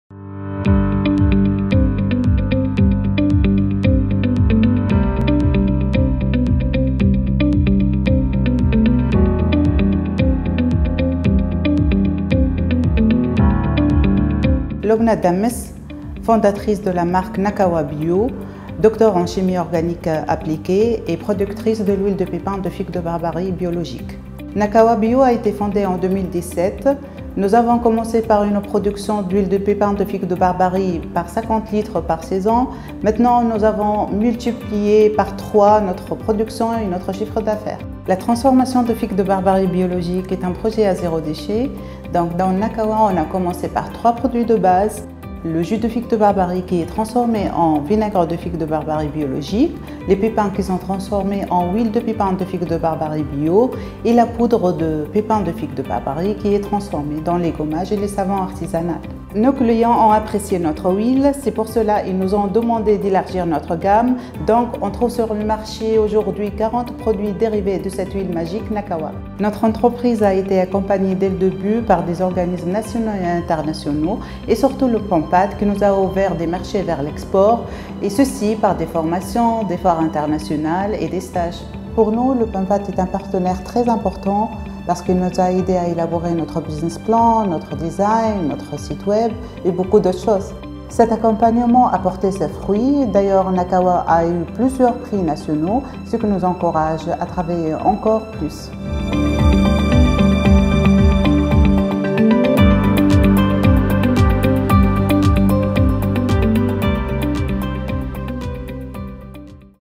PAMPAT présente NAKAWA
Témoignage de la startup Nakawa, appuyée par le projet PAMPAT